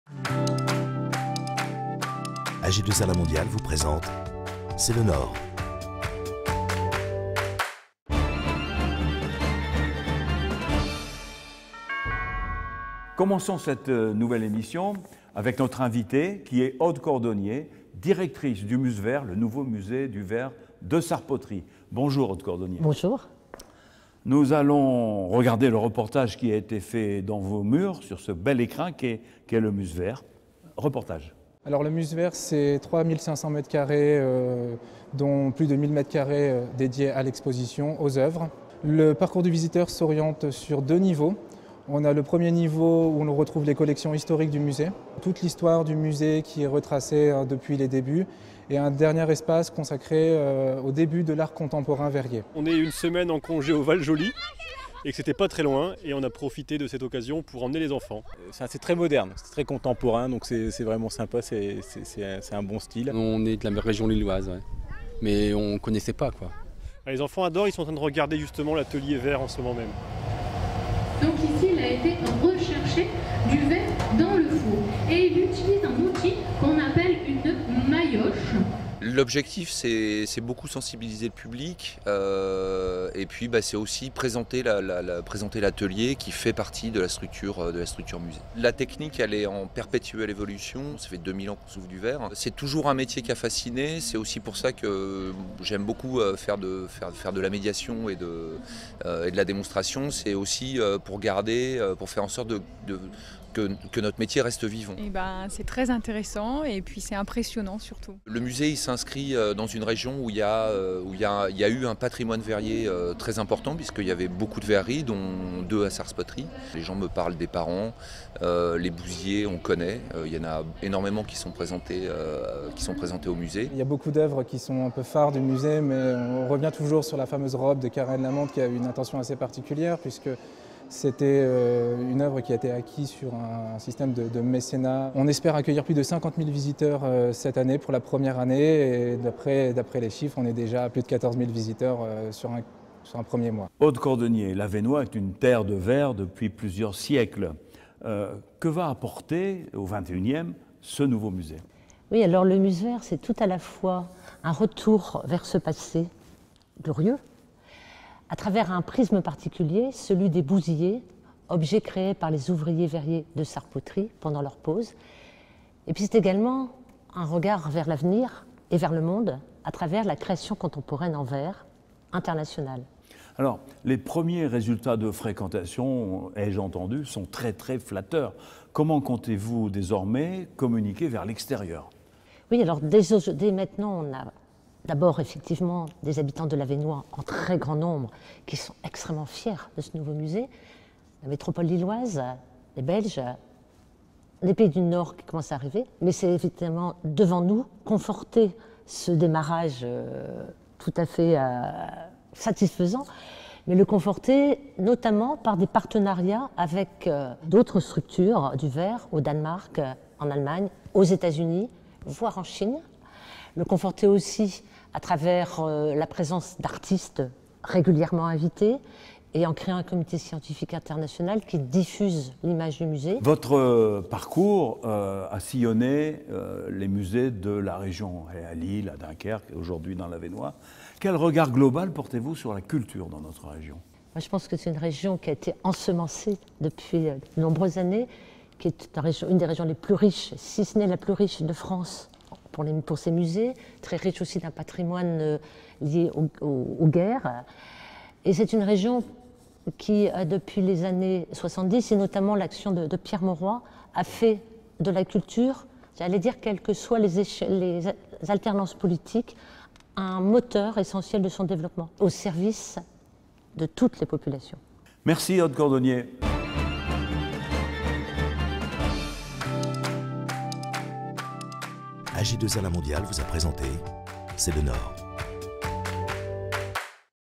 C'est le Nord: le Musverre
Entretien avec Aude Cordonnier, directrice du Musverre.